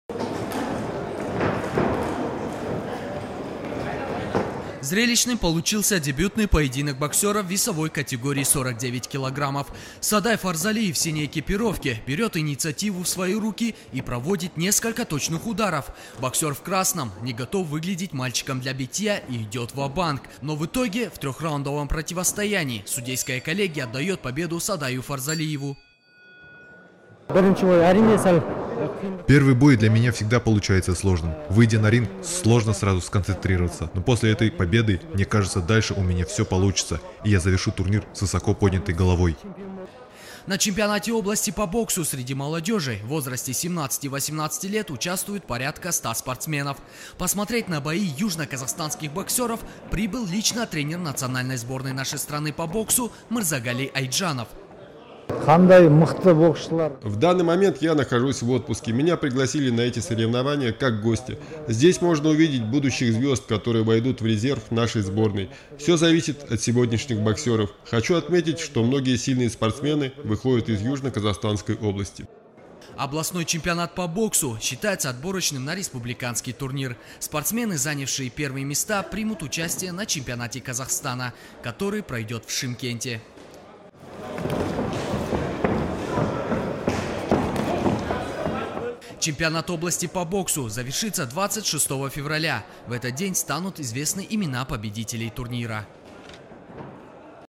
В зале центрального стадиона имени Кажимукана стартовал чемпионат области по боксу
В зале центрального стадиона имени Кажимукана стартовал чемпионат области по боксу среди молодежи.В соревнованиях принимают участие около сотни спортсменов.Посмотреть на схватки южан прибыл тренер национально сборной Казахстана